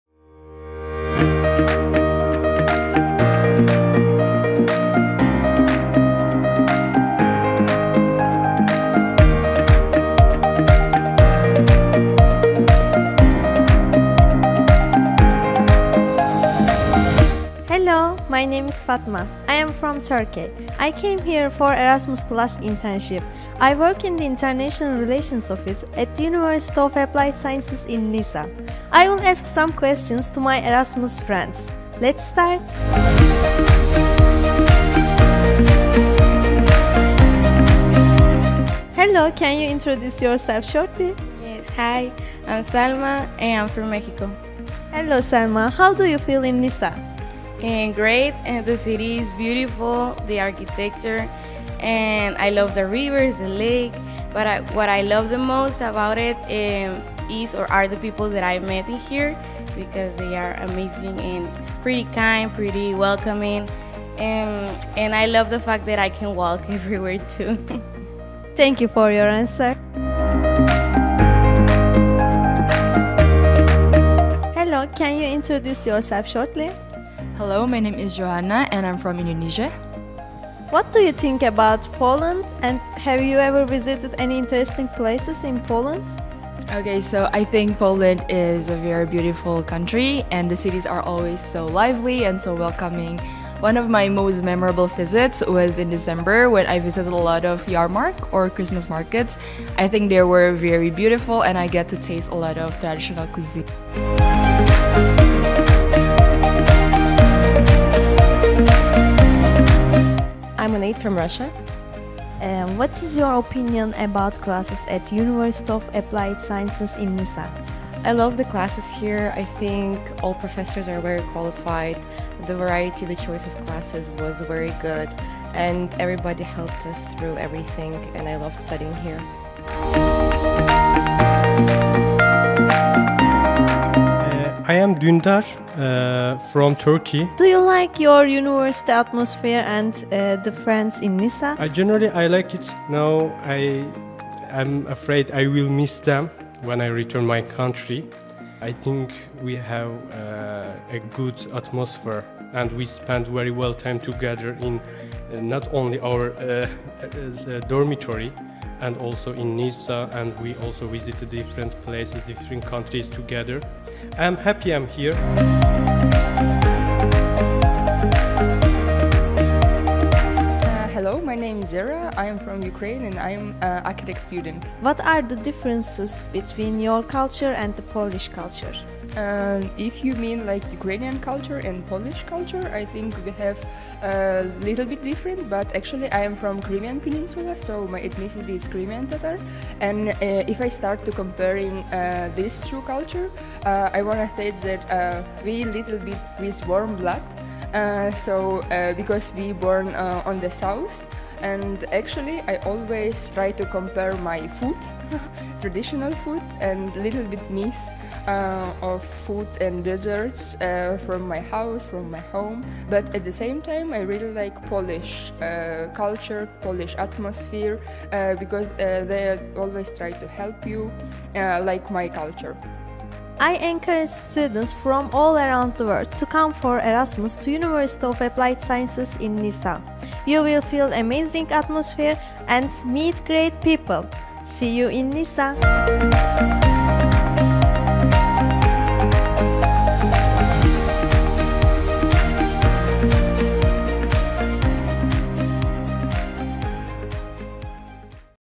International students talk about studying at the University of Applied Sciences in Nysa
"International students talk about studying at the University of Applied Sciences in Nysa and their stay in Poland"

In the winter semester 2021/22, University of Applied Sciences in Nysa hosted international students from Indonesia, Mexico, Russia, Kosovo, Turkey, Spain, Ukraine, Morocco and Albania. They realized Erasmus+ mobility in the fields of management and production engineering, English philology, finance and accounting, architecture and computer science.